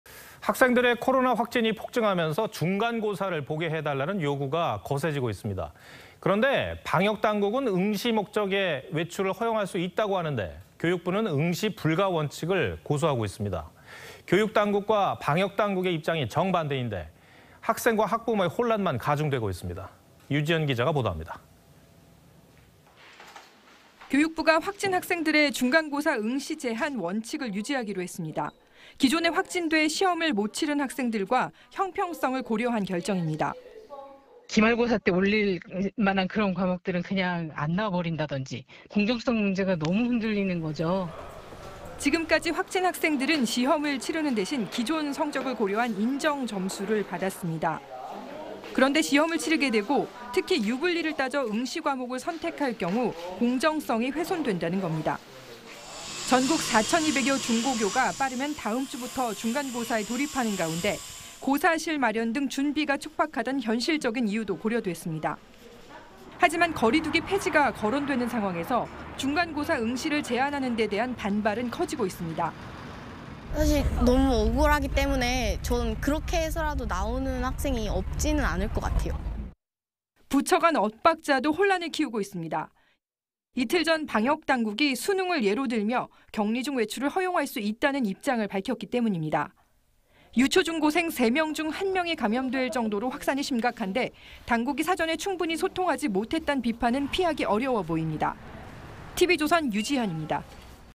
방역당국 '지원' 입장에도 교육부 "확진학생 중간고사 제한"
학생들의 코로나 확진이 폭증하면서 중간고사를 보게 해달라는 요구가 거세지고 있습니다. 그런데 방역당국은 응시 목적의 외출을 허용할 수 있다고 하는데 교육부는 '응시 불가'원칙을 고수하고 있습니다. 교육당국과 방역당국의 입장이 정반대인데 학생과 학부모의 혼란만 가중되고 있습니다.